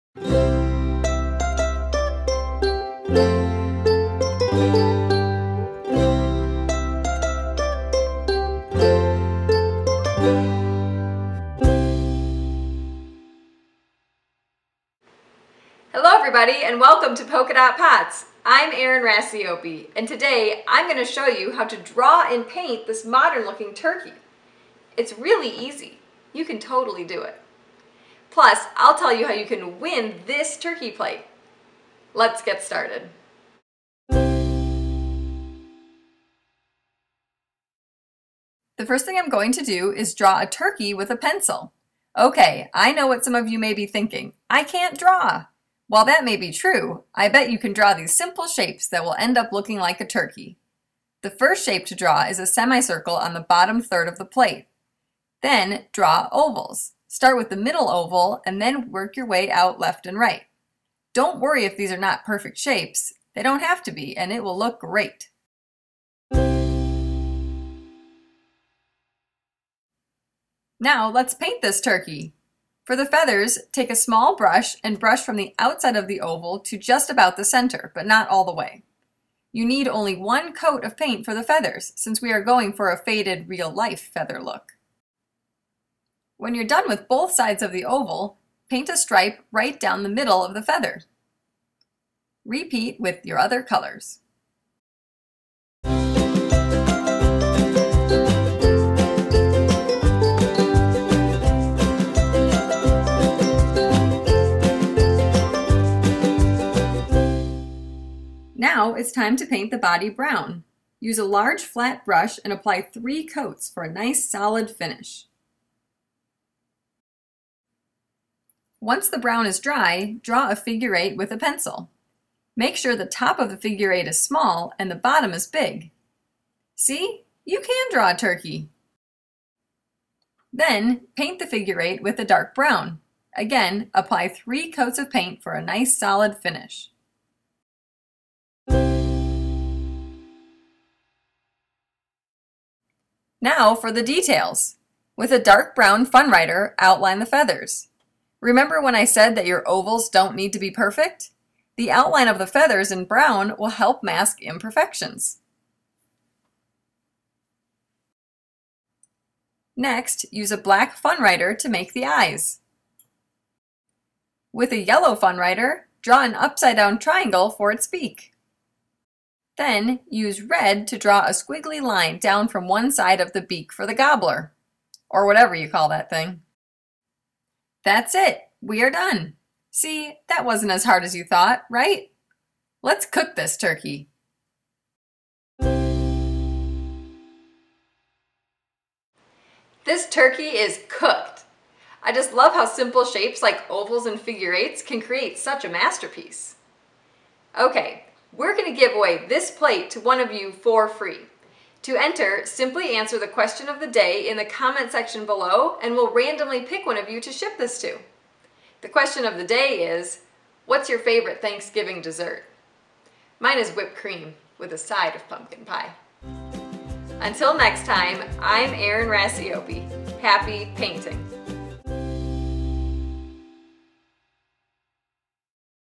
How to Paint Pottery: Modern Turkey
Learn how to paint your own pottery and ceramics.  Think you can't draw a modern turkey?  Think again!  We'll show you an easy way to draw and paint a modern looking turkey.  Use this turkey plate for the big Thanksgiving Day dinner and proudly display it the rest of the year!

Polka Dot Pots, located in Wisconsin Dells, Wisconsin and Sevierville, Tennessee has techniques that are so easy, anyone can create a masterpiece.